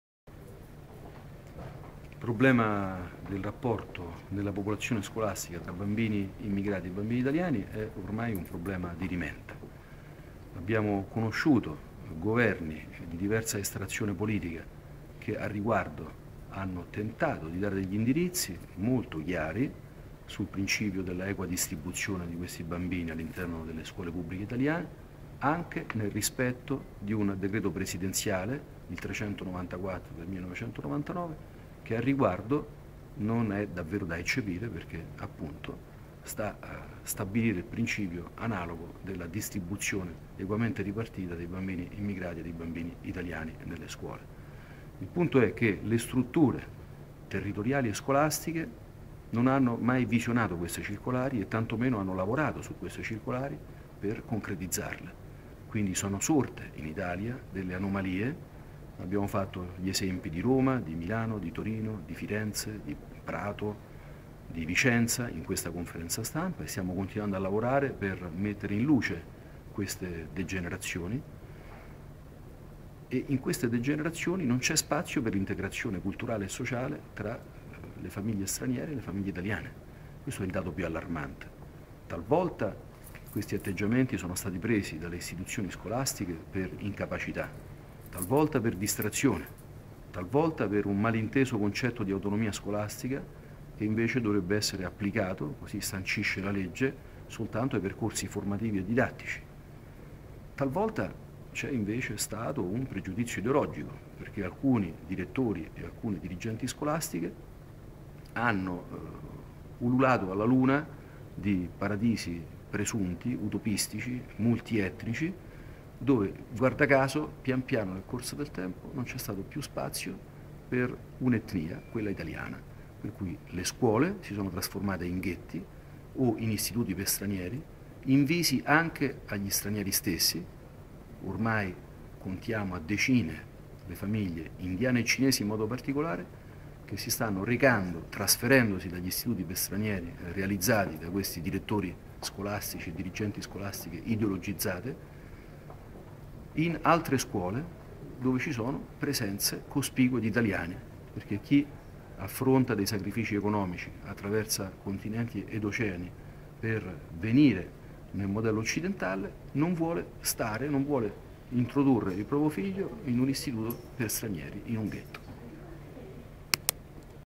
RAMPELLI SU IMMIGRAZIONE E SCUOLA
Le proposte dell'on. Fabio Rampelli sull'integrazione dei bambini stranieri a scuola